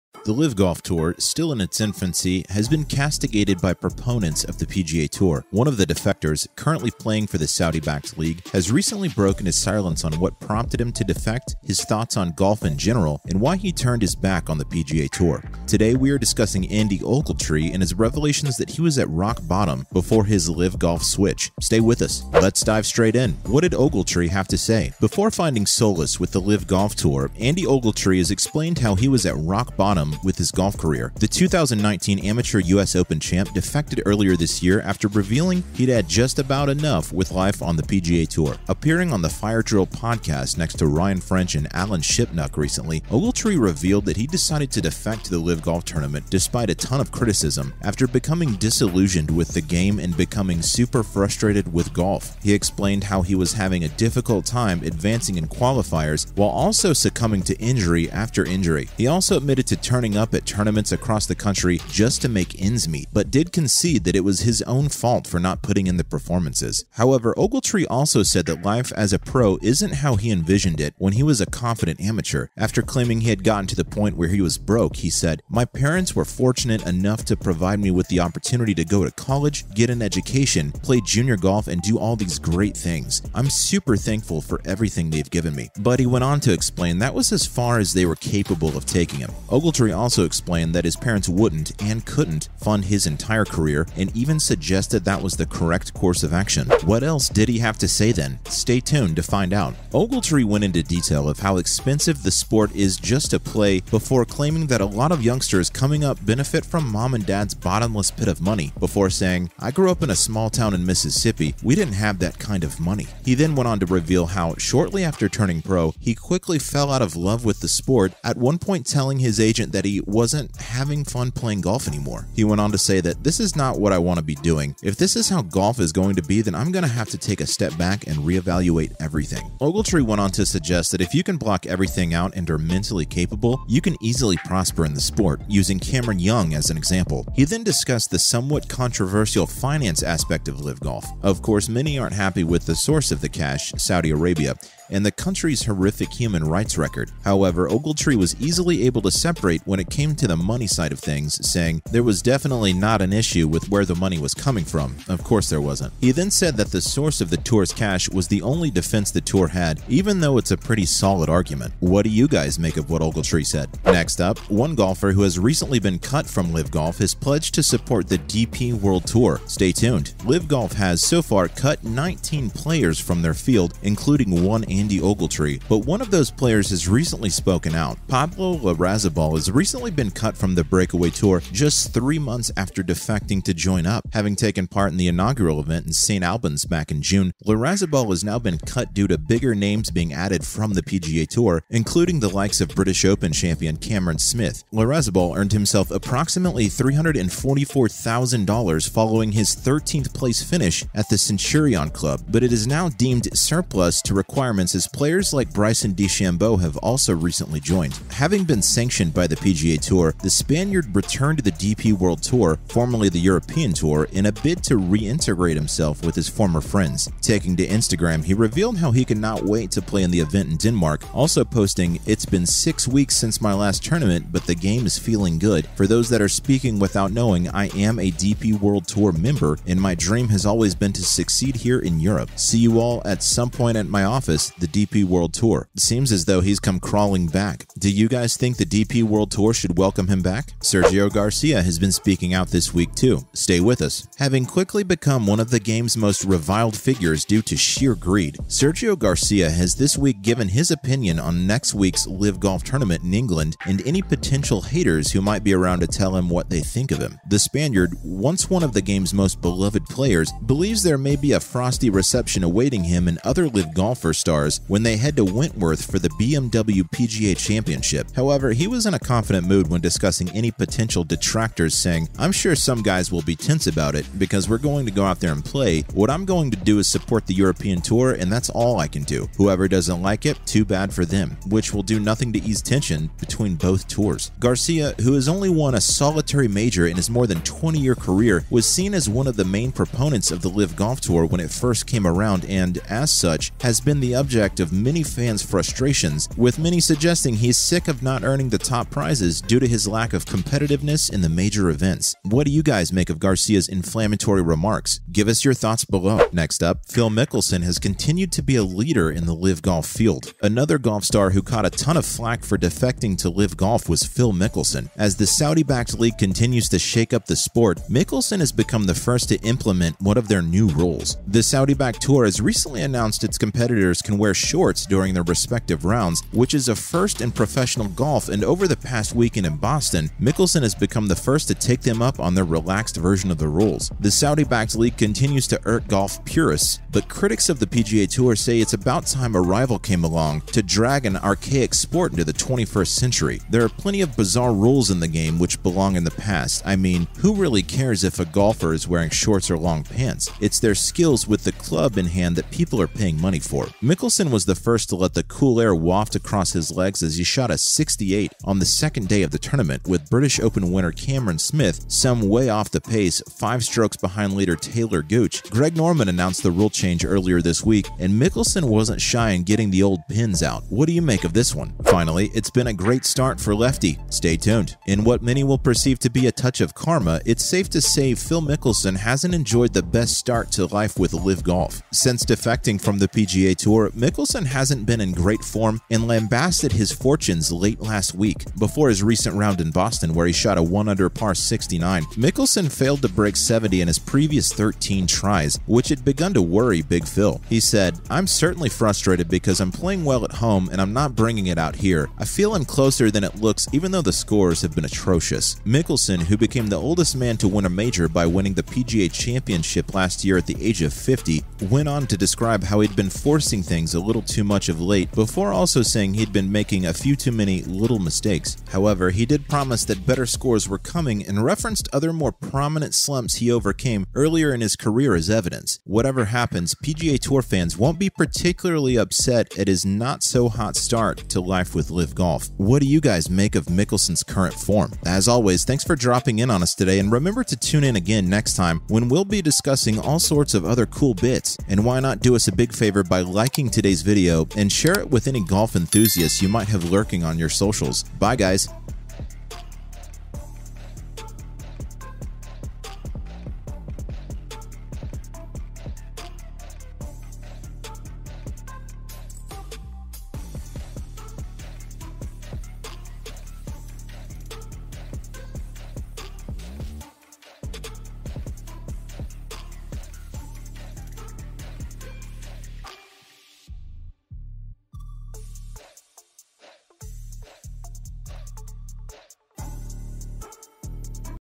Andy Ogletree OPENS UP About LIV Golf Tour Abuse..
Welcome back to Sporting Central. For the channel today, we will talk about Andy Ogletree opens up over Liv Tour Abuse + LIV Golf hidden issues. The 2019 U.S. Amateur Champion, Andy Ogletree revealed that he hit “rock bottom” just before playing the first ever LIV Golf Tour event in London.

Speaking to the Fire Drill podcast, the 24 year old Andy Ogletree revealed why he decided to join the LIV Golf despite of it receiving so much criticism and controversies ahead of its premiere season. Here’s more of Andy Ogletree opens up over Liv Tour Abuse + LIV Golf hidden issues .